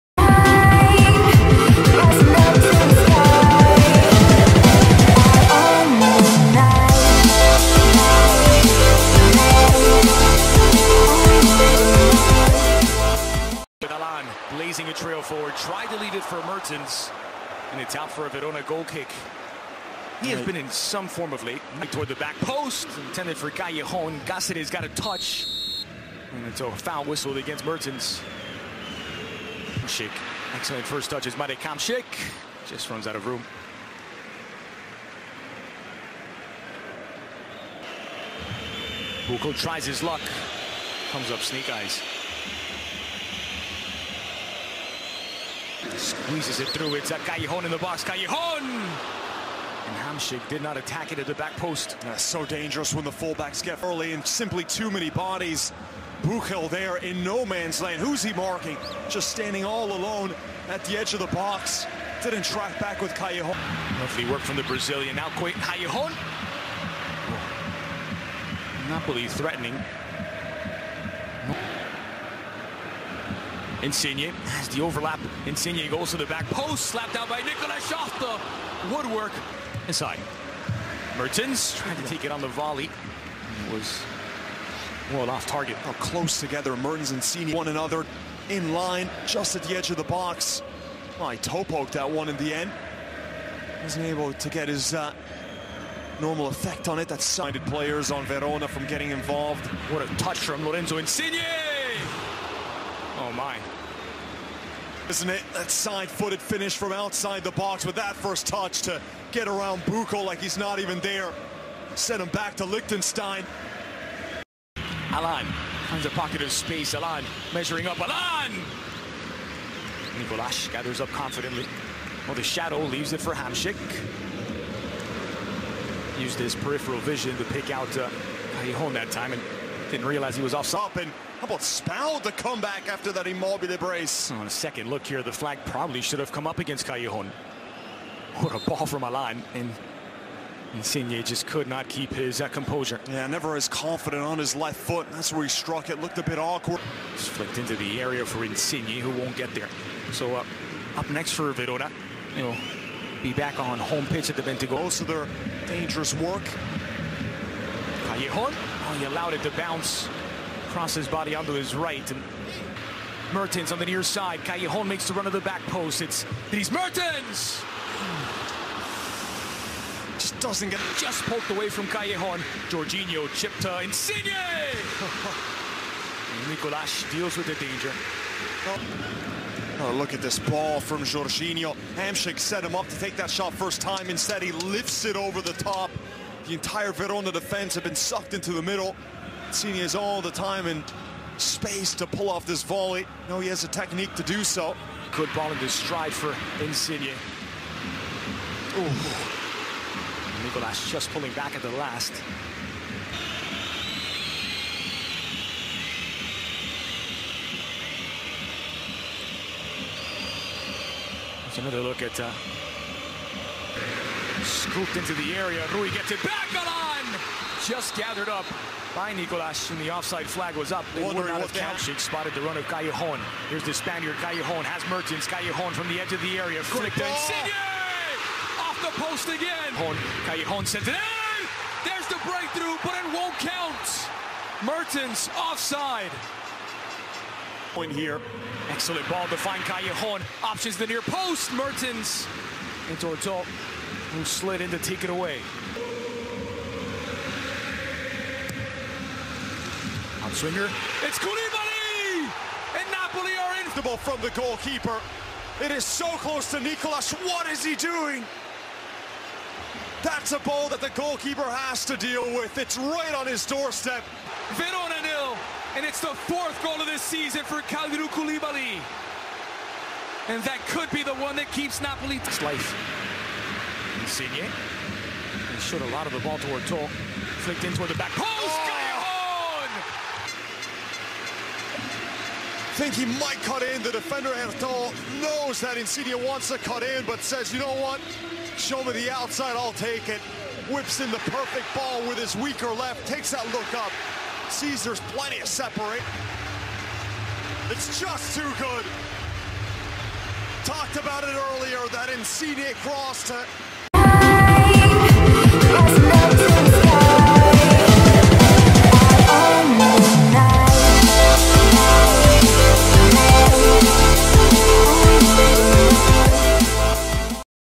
Napoli vs Hellas Verona 2-0 All Goals & Highlight SERIE A 06/01/2018
Napoli vs Verona 2-0 All Goals & Highlight SERIE A 06/01/2018

By: Corner Kick™

Goal:
65' K. Koulibaly
78' J. Callejon